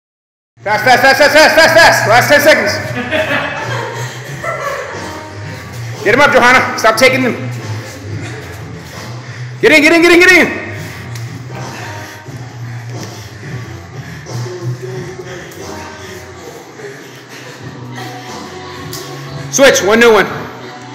TRBFH-AVLG: Andy vs Johanna - Close Quarters Standing Touch Bout
Hands up a little more Johanna. Andy, you took it easy here. Full Disclosure, I started videoing a little late. My bad. Ninja no good.